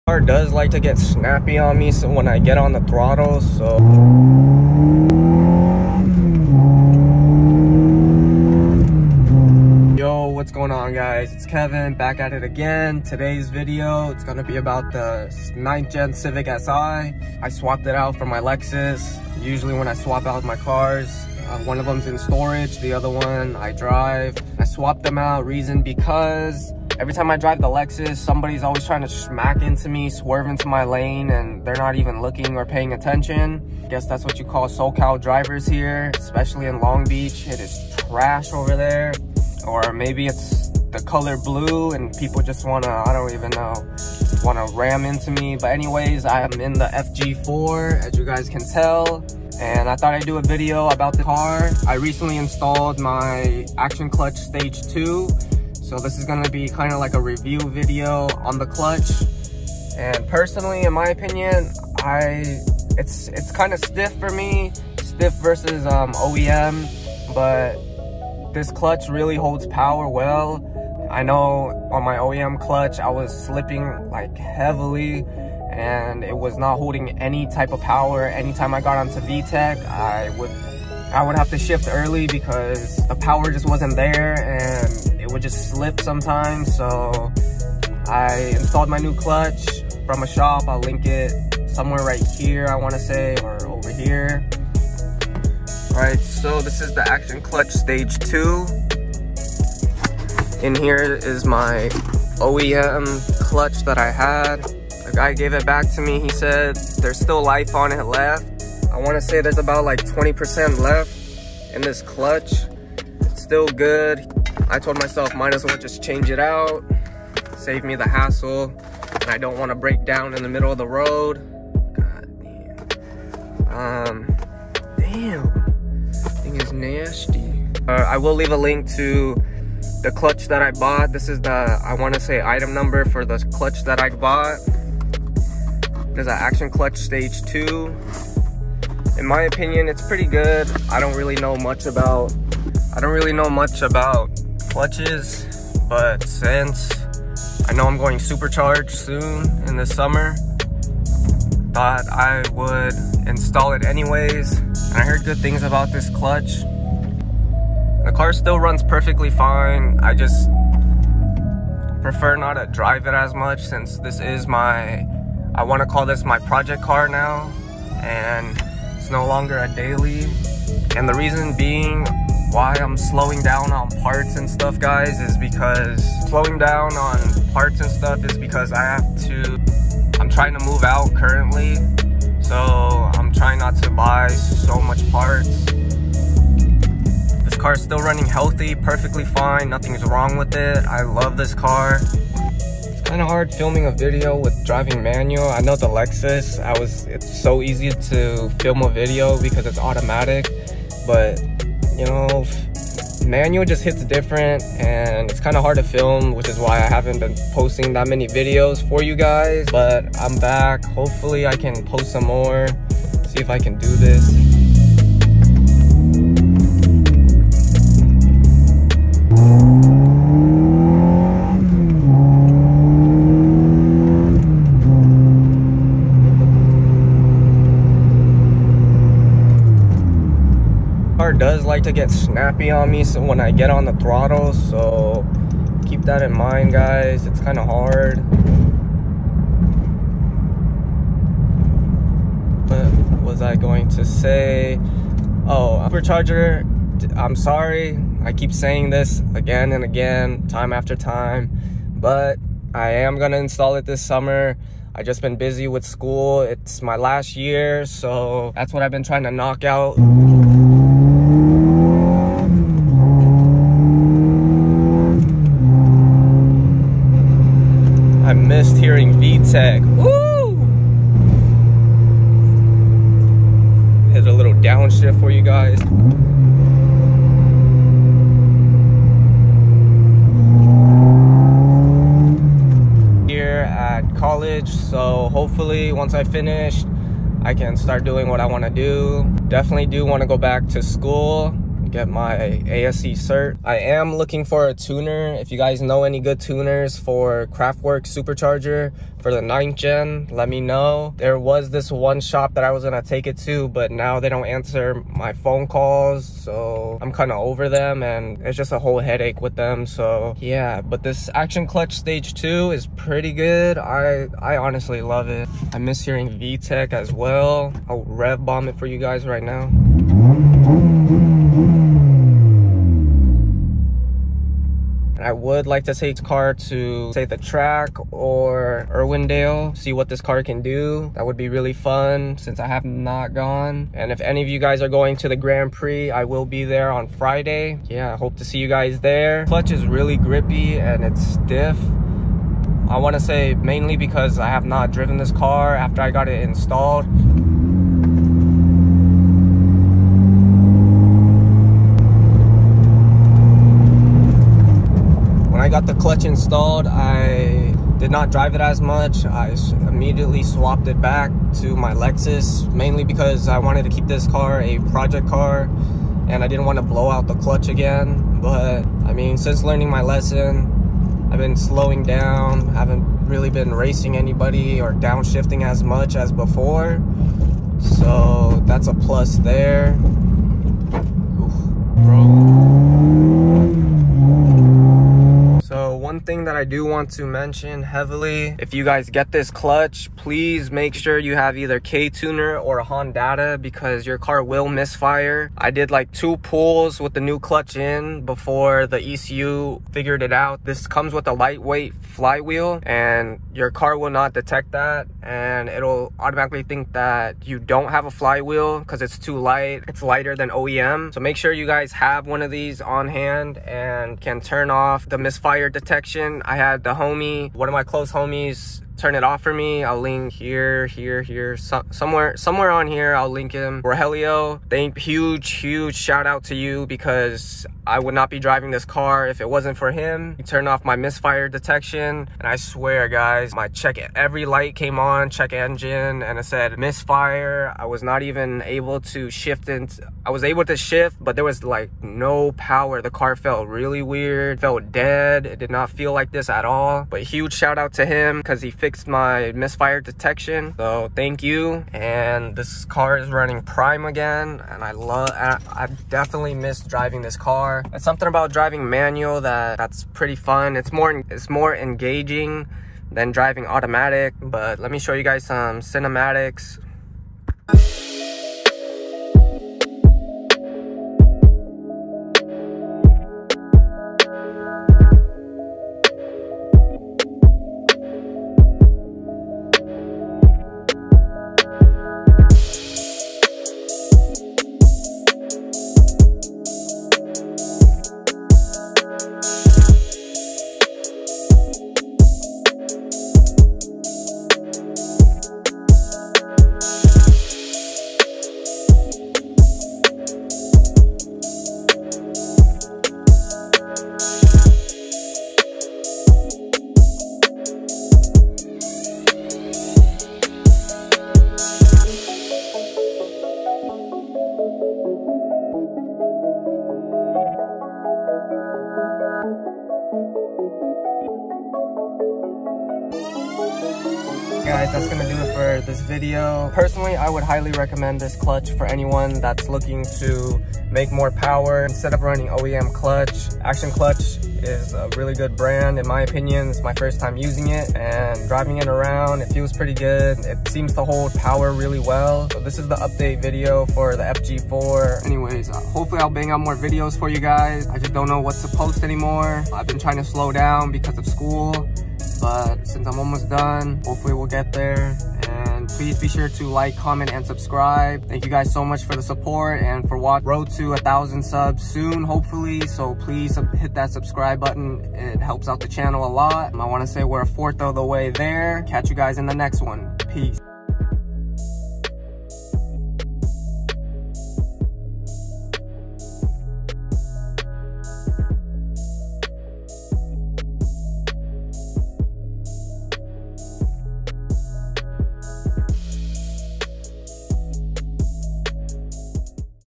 ACTION CLUTCH STAGE 2 FOR 9TH GEN CIVIC SI/FG4 ! UPDATE VIDEO !
In this video I’m going to review action clutch stage 2 for the 9th gen civic si/ fg4. I recently got it put on and I have a good feeling about this clutch kit. A little update video on the civic since I have not made any content about this car in awhile. I hope y’all enjoy catch you in the next one!

Thank you everyone for supporting and watching ! As always stay safe out there !